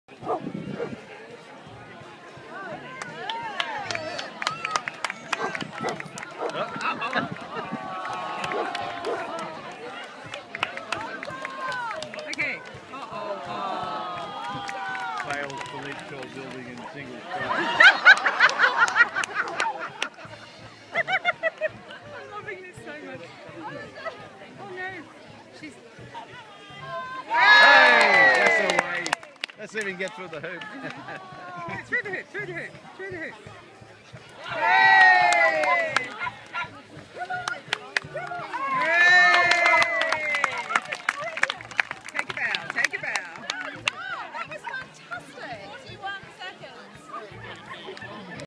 The Donhead St Andrew Fete - the Dog Show
One competitor in the doggie obstacle course